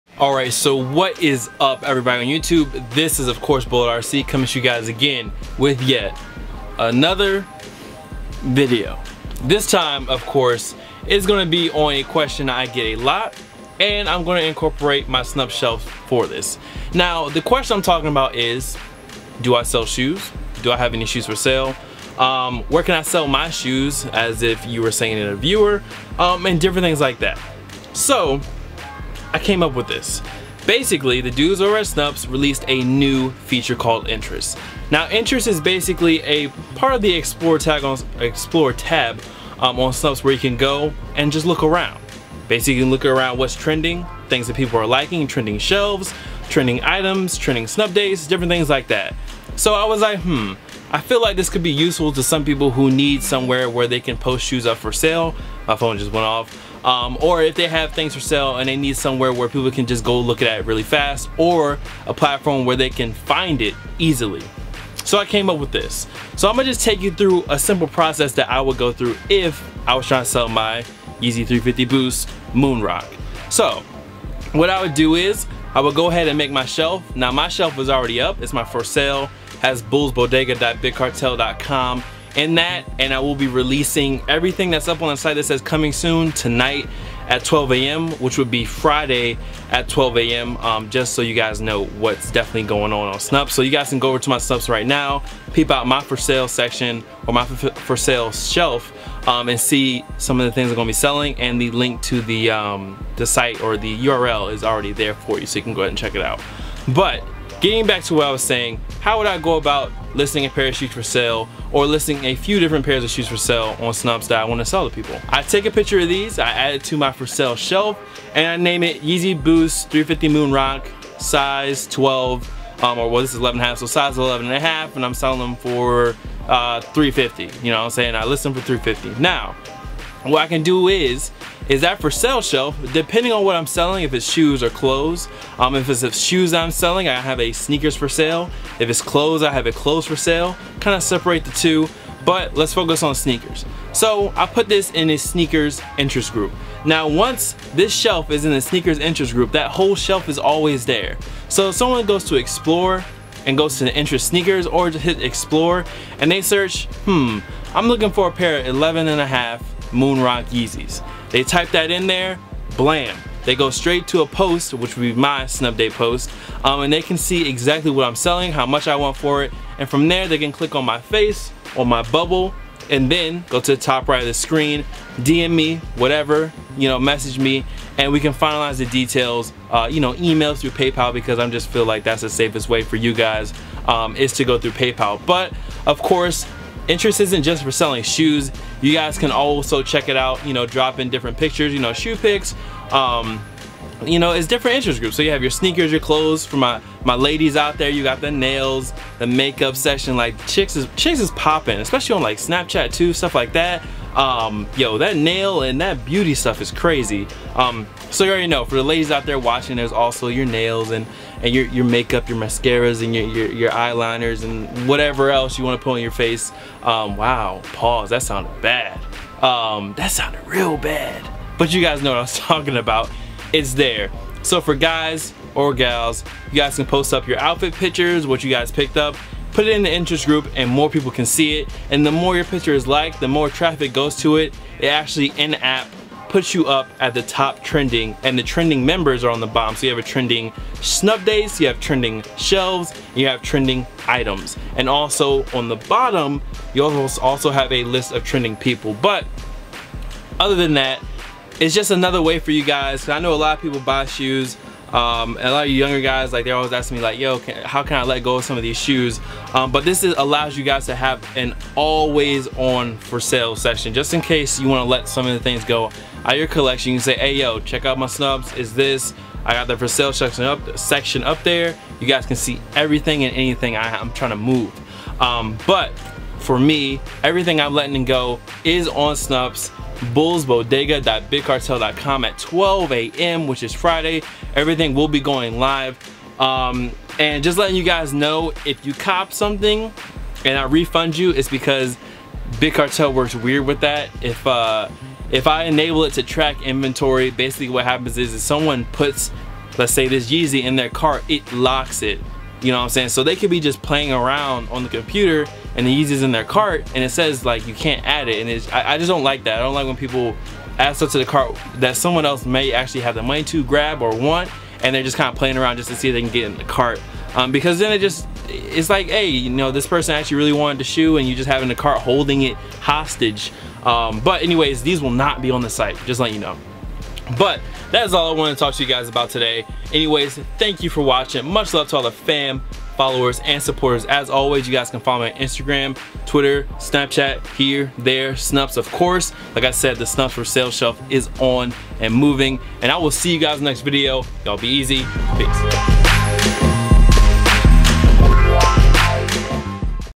Do I Sell Shoes!? Bulls Bodega Restock!
Since I Get Asked If I Sell Shoes - "Where Can I Sell Shoes" - "Blahzay Blah" I Figured I'd Answer That's Question With A Little Help From Snupps. ENJOY! & The Site Restocks At 12!